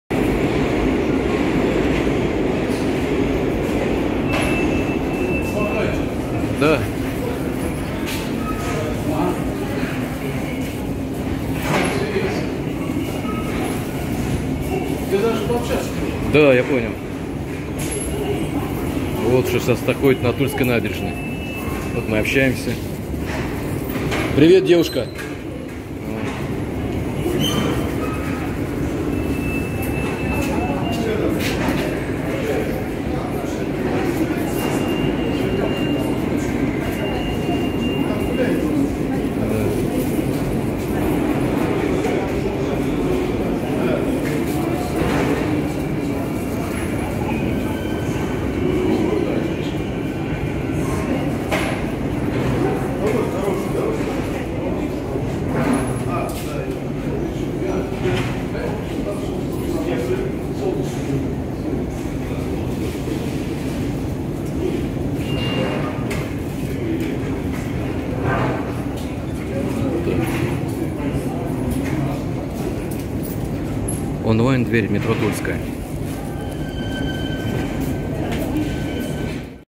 Онлайн-дверь Москва-Тула. Туляки открыли портал!
Москва. Метро Тульская. Открытие онлайн-двери Москва-Тула в метро.